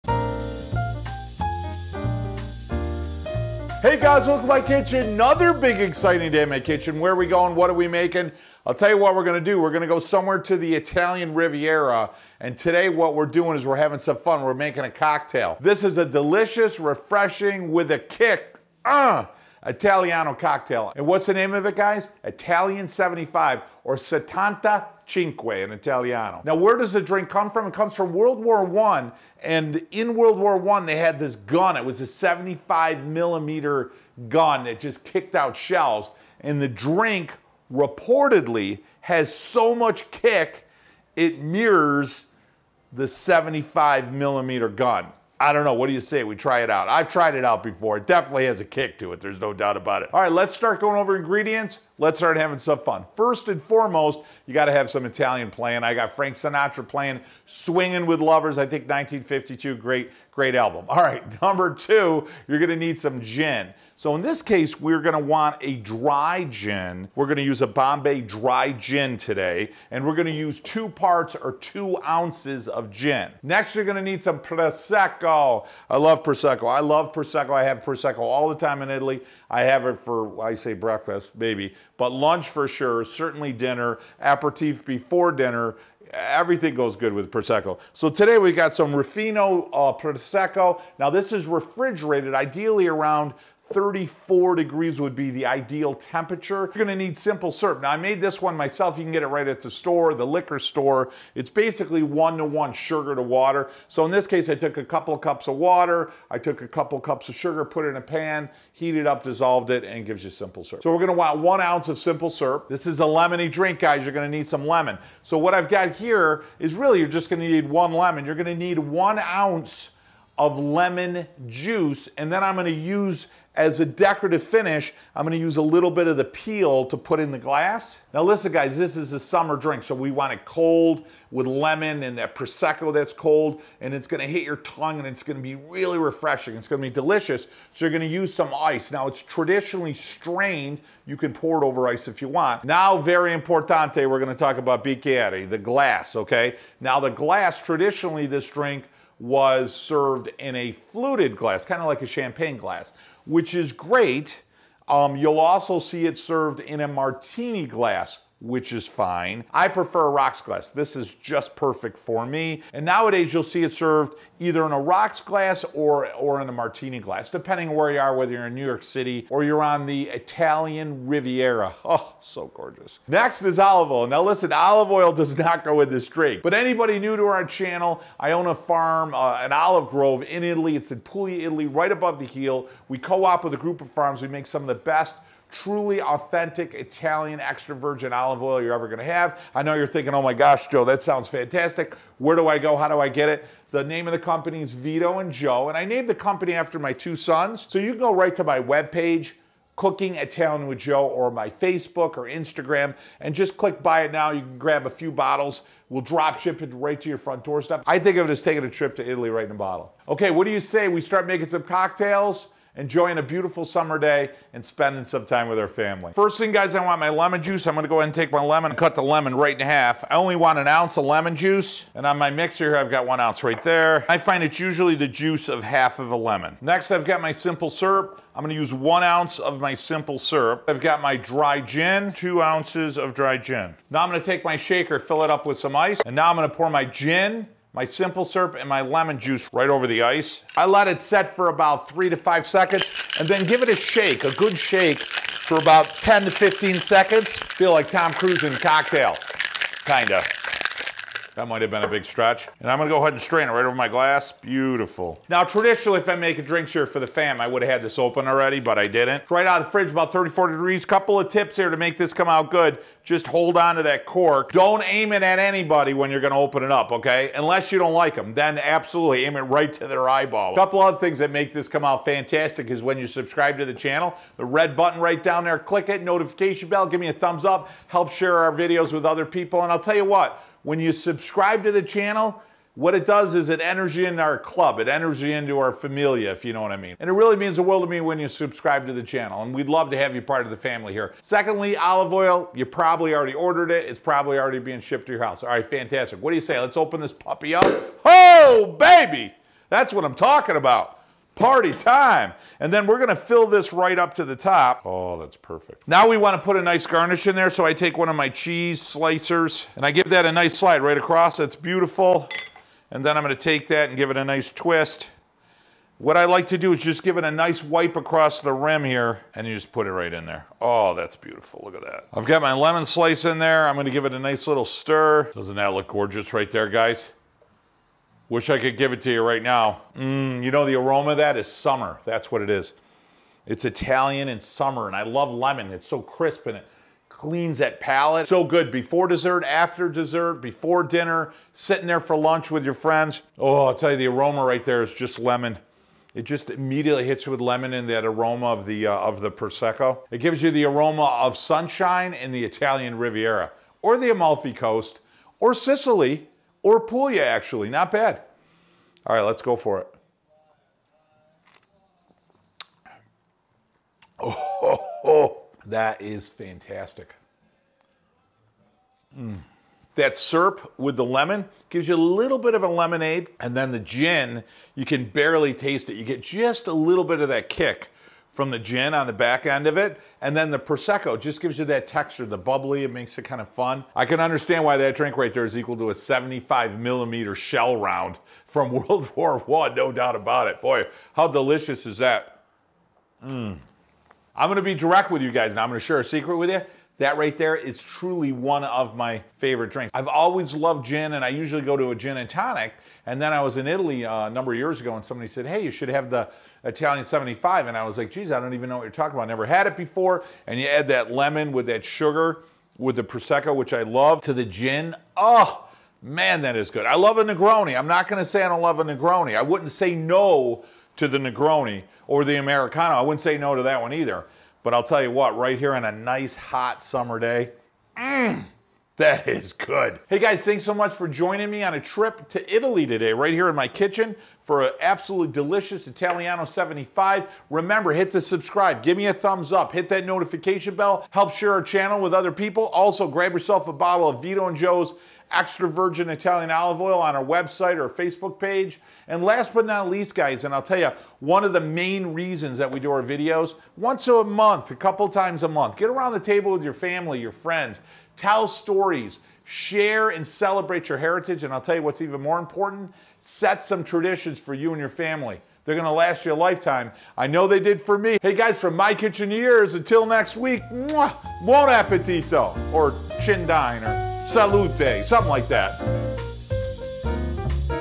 "The Italian 75" Cocktail | Cooking Italian with Joe
❤️🇮🇹💋🍷 Join me today in the kitchen today as we head to the Italian Riviera to enjoy a delicious Italian cocktail, the Italian 75!!! A cool, delicious, and refreshing drink made with Prosecco, Dry Gin, Lemon juice, simple syrup and ice. Great Italian drink to enjoy on a hot summer day.